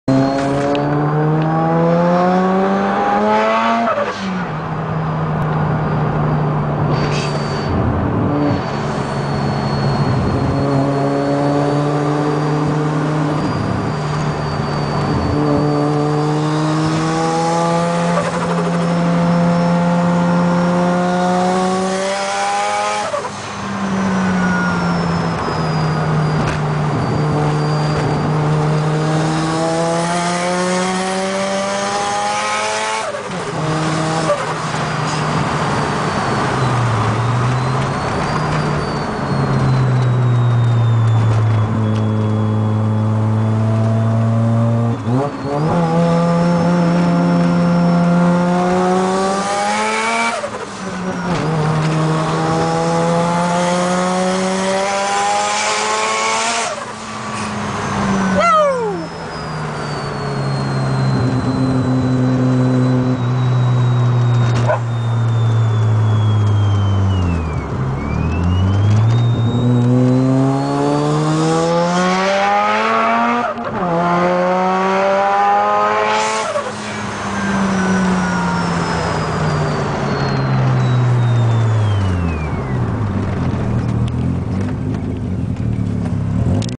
Zoom Zoom
Speeding through the streets of Anguilla!